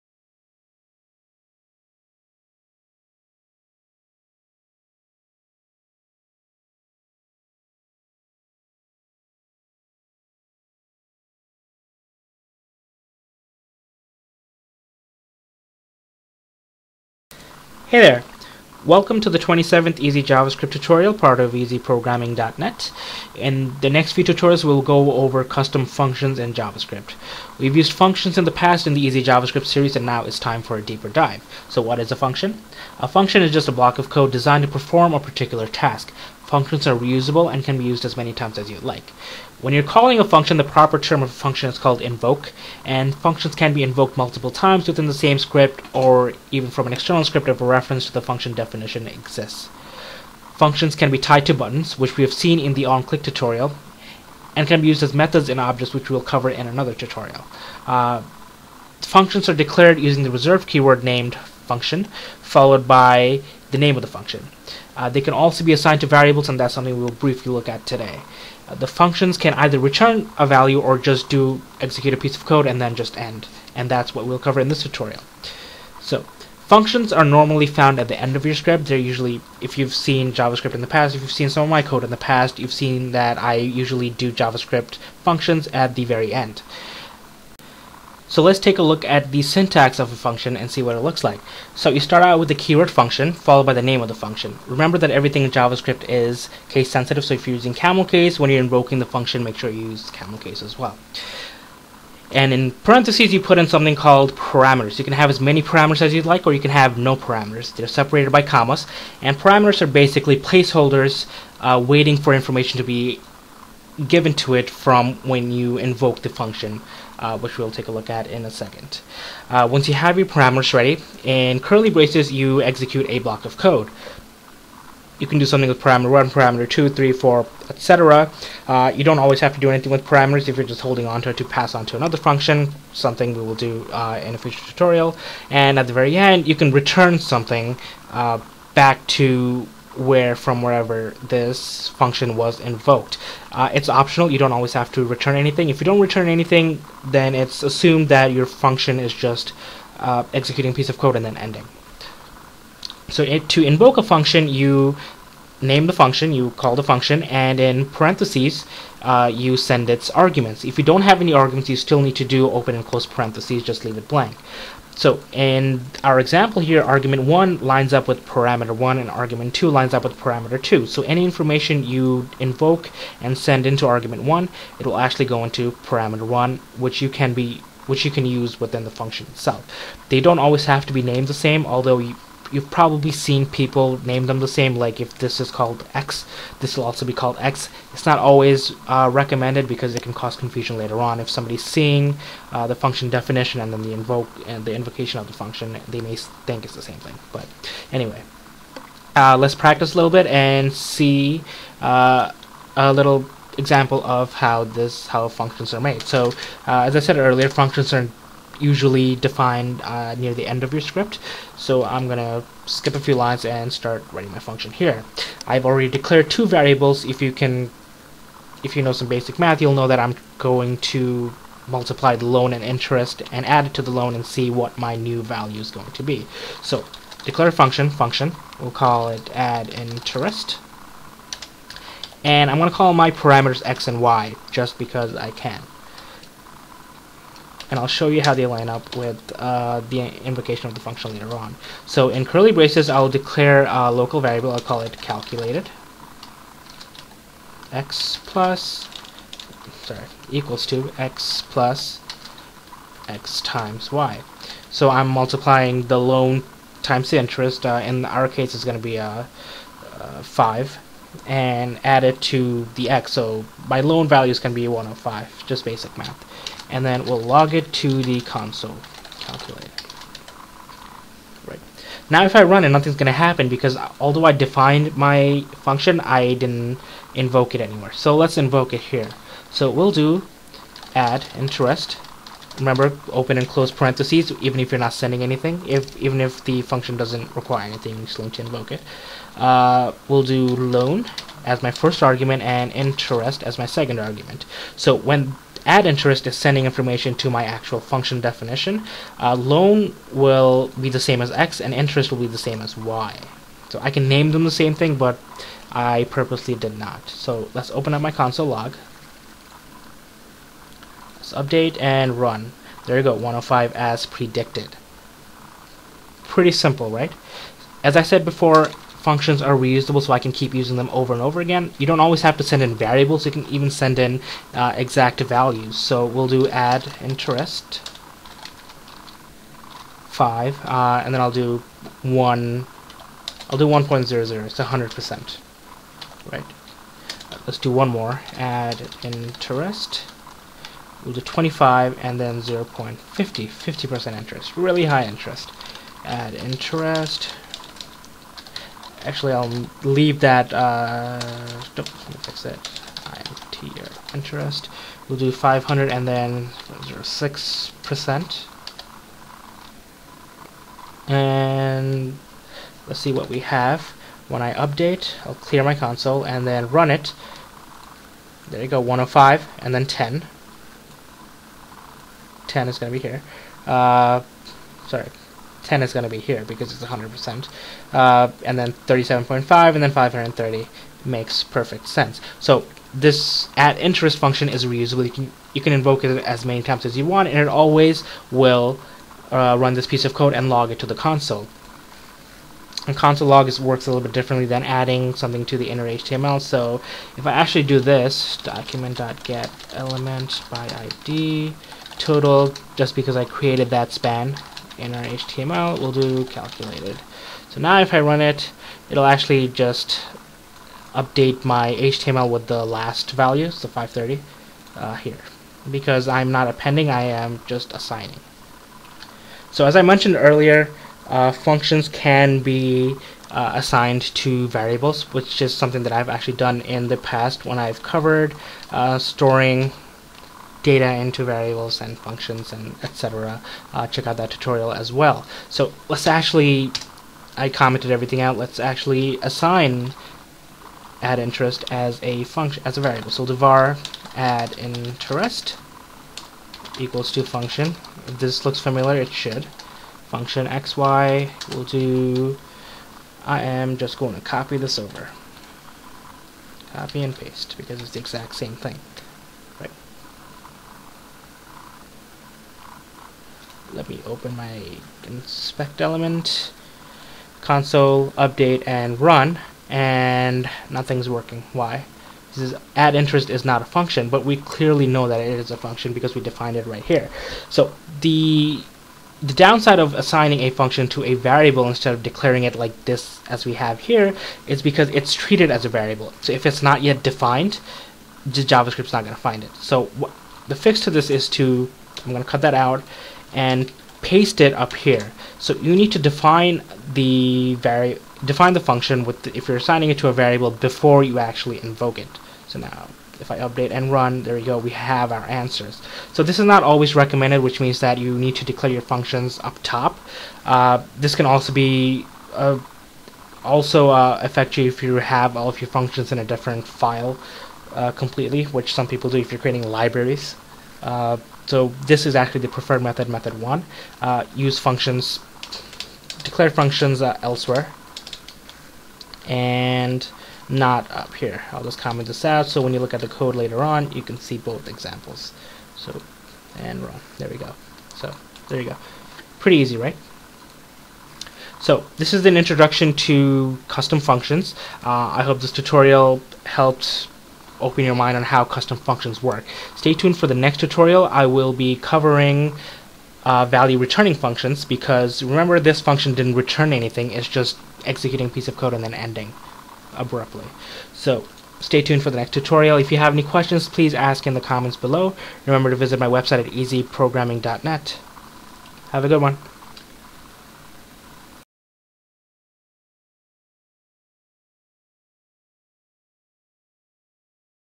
Easy JavaScript - Intro to Custom JavaScript Functions (27)
Welcome to the 27th Easy JavaScript tutorial, part of EasyProgramming.net. I apologize for the static in this video! I've gotten a new mic since this was recorded.

The next few tutorials will go over custom functions in JavaScript. We've used functions in the past in the Easy JavaScript series, and now it's time for a deeper dive.

A function is just a block of code designed to perform a particular task. Functions are also reusable. When you call a function, the proper term is invoking a function, and it can be invoked multiple times within the same script (or even from external scripts if a reference to the function definition exists).

Functions can be tied to buttons (which we saw in the onclick tutorial) and be used as methods in objects (something we will cover in another tutorial). They are delcared with the JavaScript reserved keyword named function followed by the name of the function. They can also be assigned to variables. And functions can either return values or just do a basic command and end.

Functions are normally found at the end of your script, or even a completely different file by referencing the file.

Parameters can be defined to receive values from another part of your script. The piece of code sending the parameter is called the argument.

to view my other programming tutorials! Find videos of C++ tutorials, Excel tutorials, and JavaScript Tutorials.

I hope you enjoy the video and if you have any questions, please ask in the comments below. If you have any requests feel free to let me know.